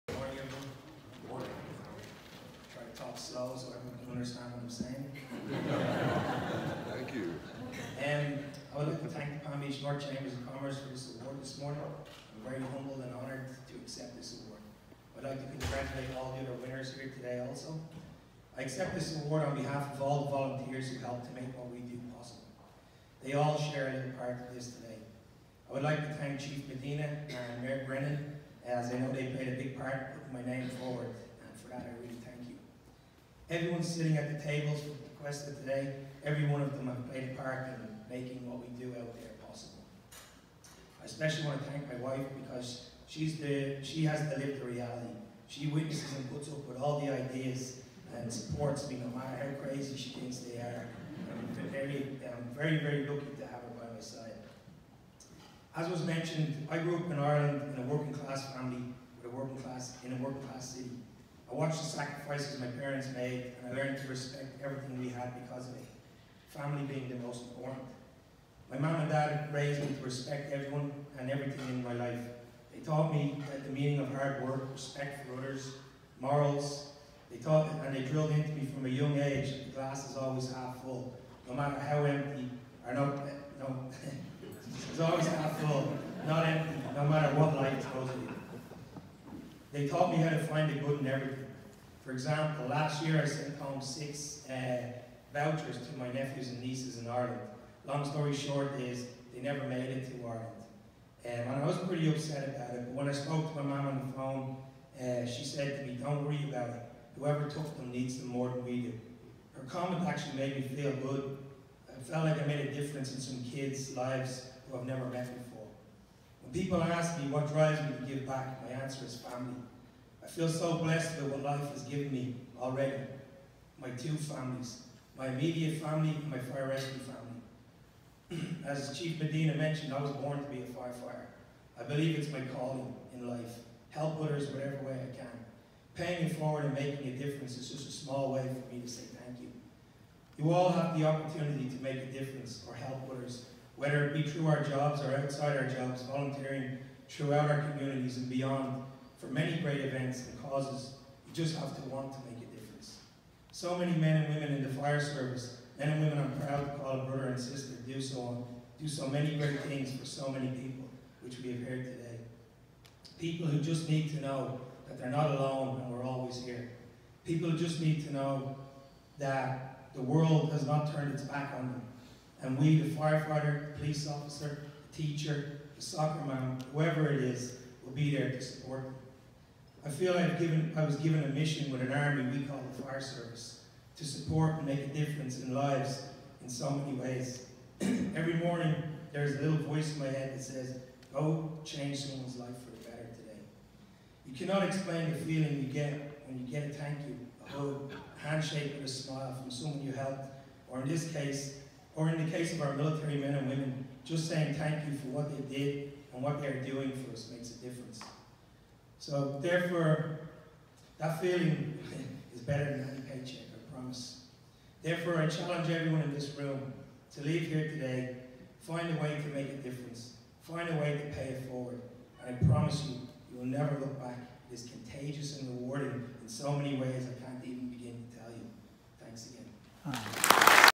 Village of Tequesta
4/18/18 - Firefighter Dave McGovern, of the Tequesta Fire Rescue Department, received the NPB Chamber's Inaugural Community Service Award for Palm Beach North First Responders.